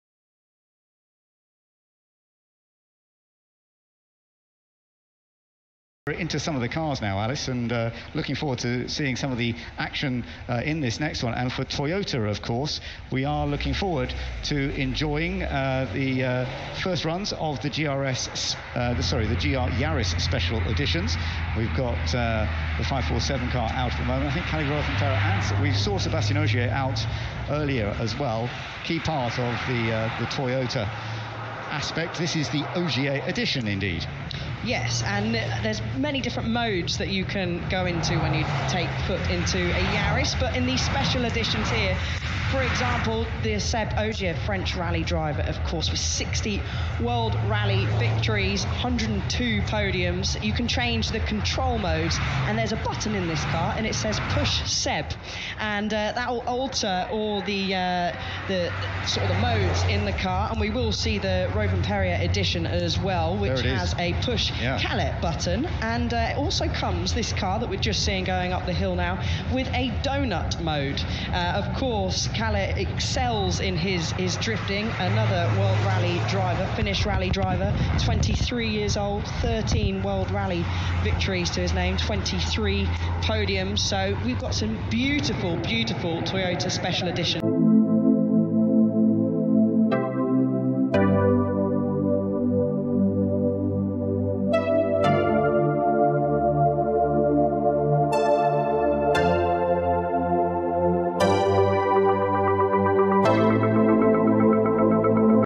Exclusive Toyota GR Yaris Editions for Rally Champions at Goodwood Festival of Speed 2024
Experience the thrill of the 2024 Goodwood Festival of Speed as World Rally Champions Sébastien Ogier and Kalle Rovanperä debut their custom Toyota GR Yaris special editions. Each limited to just 100 units, these exclusive cars reflect their namesakes' unique styles and preferences. Watch as Ogier and Rovanperä showcase their impressive rally skills, driving their strikingly different GR Yaris editions up the Hill. Don't miss this exhilarating First Glance, presented by heycar, and witness the future of high-performance rally cars.